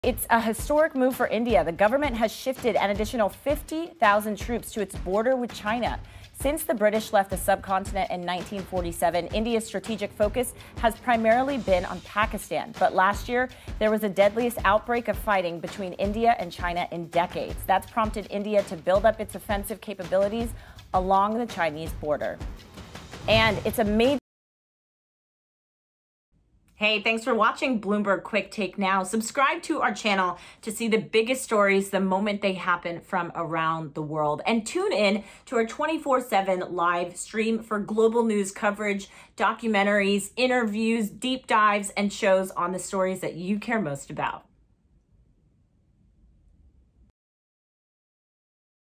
India Shifts 50,000 Troops to China Border in Historic Move
India has redirected at least 50,000 additional troops to its border with China in a historic shift toward an offensive military posture against the world’s second-biggest economy.

Although the two countries battled in the Himalayas in 1962, India’s strategic focus has primarily been Pakistan since the British left the subcontinent, with the long-time rivals fighting three wars over the disputed region of Kashmir. Yet since the deadliest India-China fighting in decades last year, Prime Minister Narendra Modi’s administration has sought to ease tensions with Islamabad and concentrate primarily on countering Beijing.

Over the past few months, India has moved troops and fighter jet squadrons to three distinct areas along its border with China, according to four people familiar with the matter. All in all, India now has roughly 200,000 troops focused on the border, two of them said, which is an increase of more than 40% from last year.